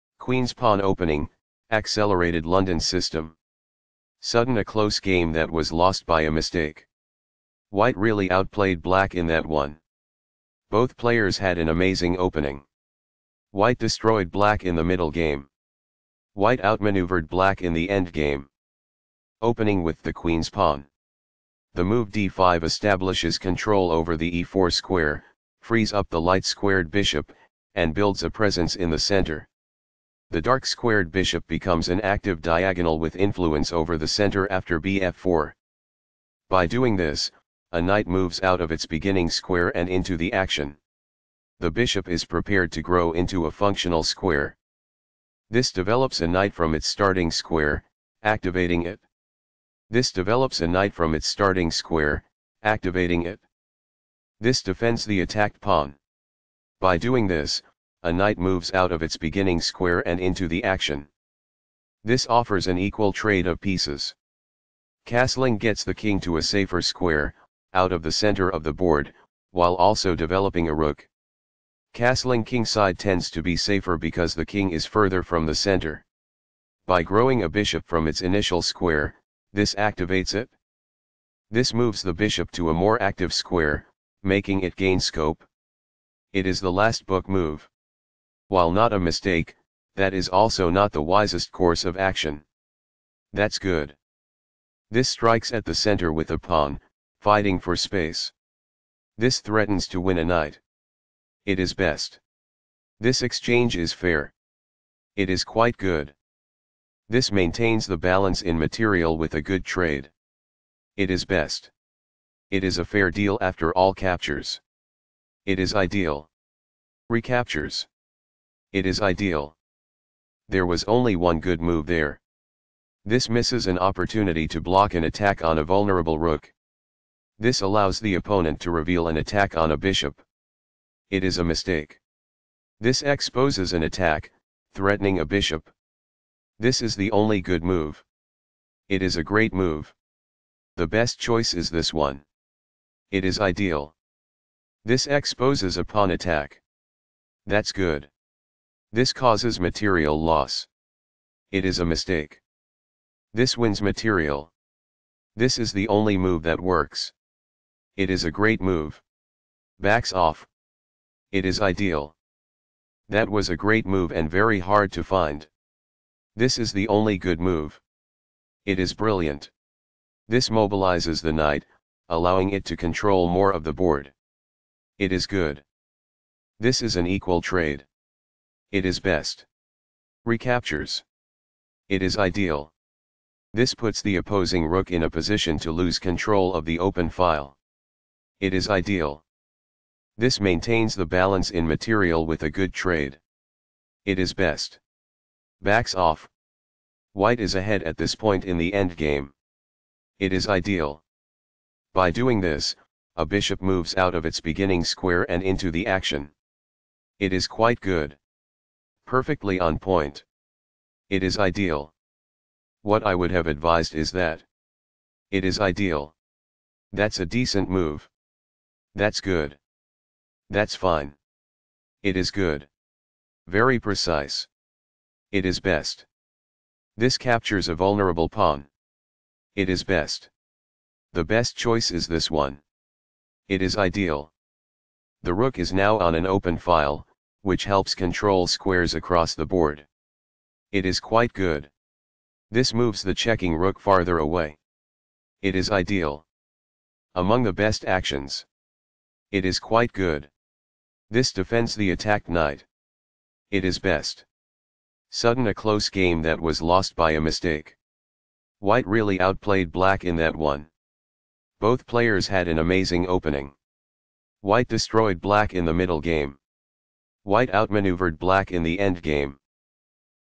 White Carlsen, M., Black Bosiocic, Mari,Queen's Pawn Opening: Accelerated London System, Event Worl
Queen's Pawn Opening: Accelerated London System, Event World Blitz 2016, Site Doha QAT, Date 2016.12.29, Round 12.1, White Carlsen, M., Black Bosiocic, Mari, Result 1-0, ECO D00, WhiteElo 2840, BlackElo 2591, PlyCount 49, EventDate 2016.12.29, EventType swiss (blitz), EventRounds 21, EventCountry QAT, SourceDate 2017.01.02